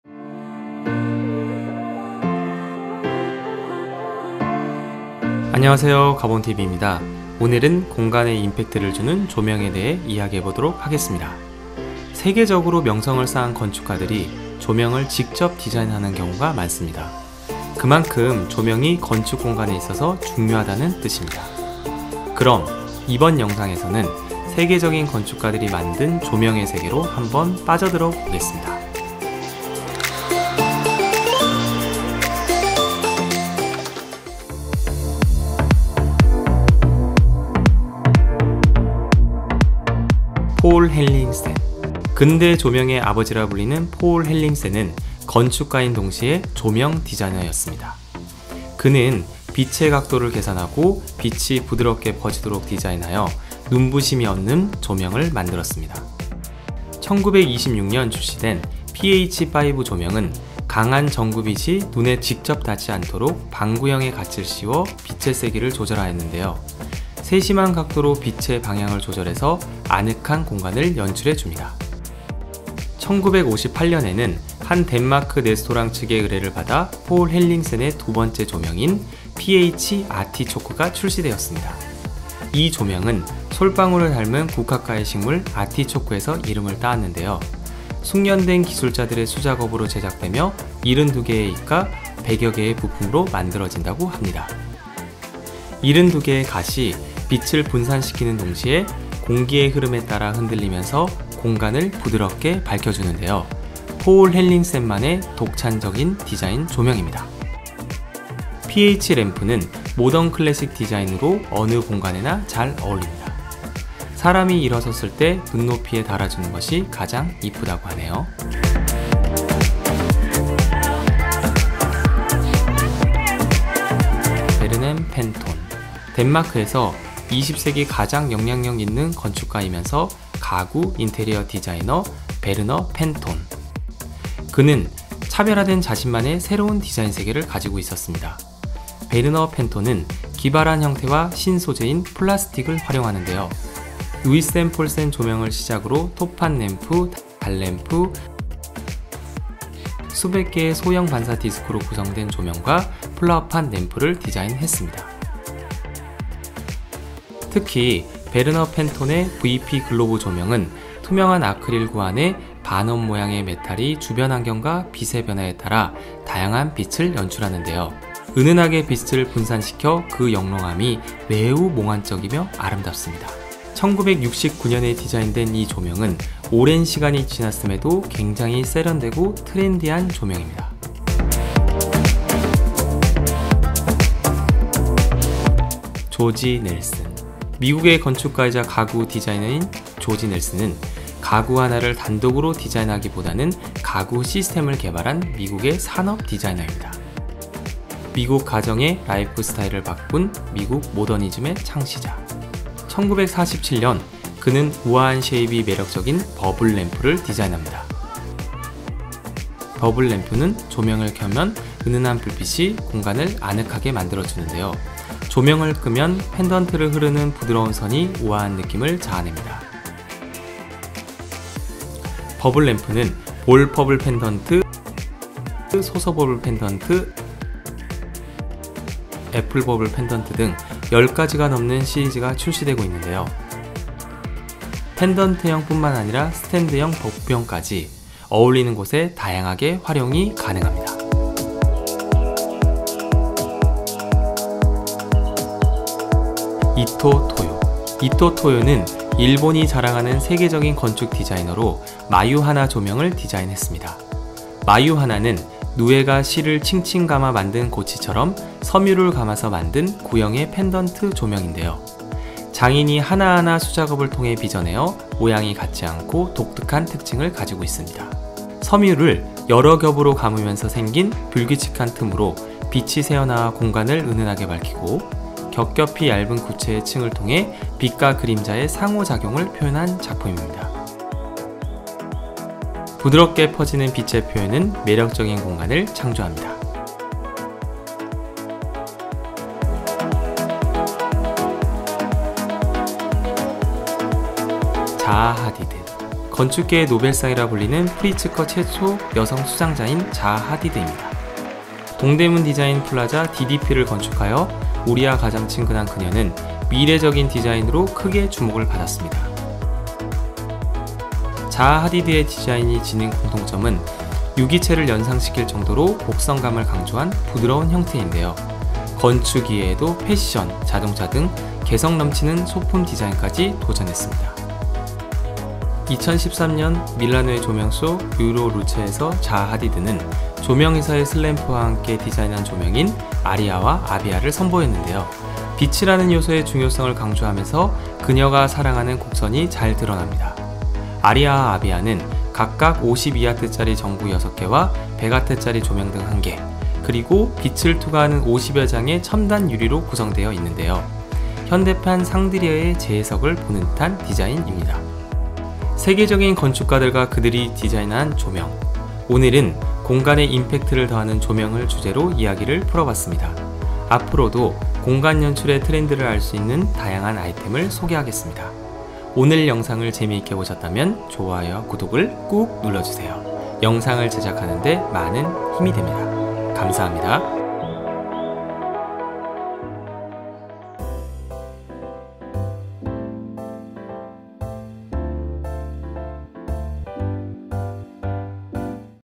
공간에 임팩트를 더하는 조명 TOP5 ! 세계적인 건축디자이너가 디자인한 조명 | Interior Design Lighting
안녕하세요. 가봄TV입니다.
오늘은 공간에 임팩트를 주는 조명에 대해 이야기를 해보겠습니다.
세계적으로 명성을 쌓은 건축가들이 조명을 직접 디자인 하는 경우가 많습니다.
그만큼 조명이 건축공간에 있어서 중요하다는 뜻 입니다.
그럼 이번 영상에서는 세계적인 건축가들이 만든 조명의 세계로 한번 빠져들어 보겠습니다.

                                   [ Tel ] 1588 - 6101
라이프스타일에 필요한 모든 곳을 가봄! '가봄TV'